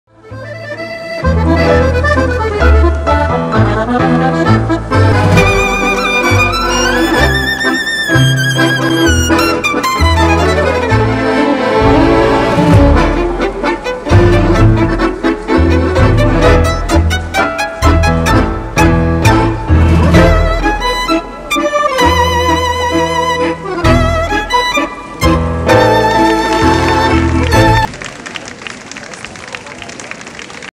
Momentos intensos com Kazaar e Dharkan -- Tango
Assista aos vários momentos intensos que a Nespresso proporcionou a quem passeava no Chiado. Saiba mais sobre os Grands Crus Kazaar e Dharkan em: